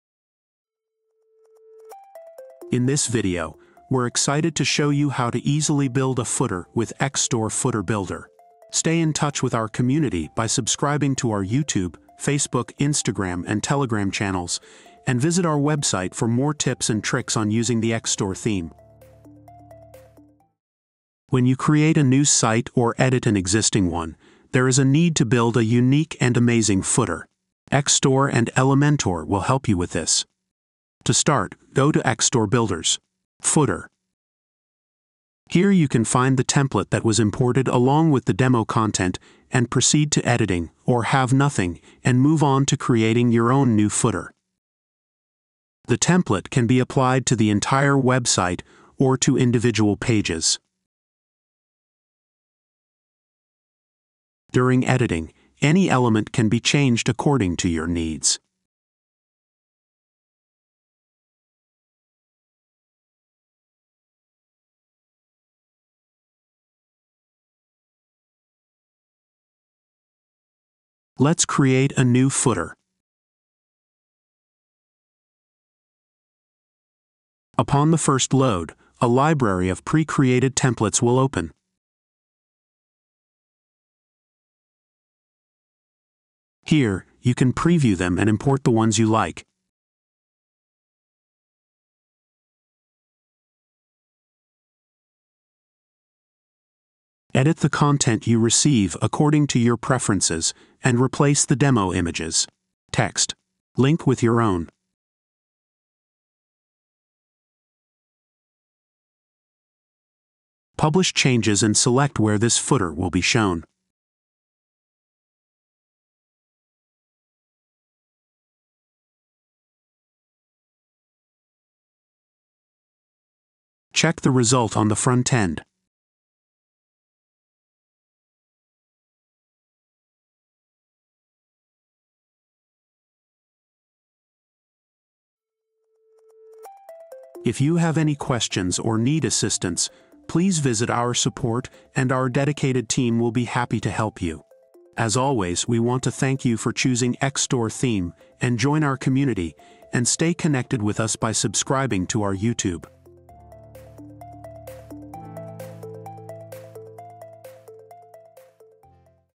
[14] Create Website Footer Using XStore Footer Builder with Elementor & WordPress
Learn how to create a stunning website footer using XStore Footer Builder with Elementor & WordPress! In this tutorial, we'll guide you through the process step-by-step to design and customize your footer to perfection. Don't miss out on this opportunity to enhance the look and functionality of your website. Watch now and take your website design to the next level with XStore!

Follow us on all your favorite social networks.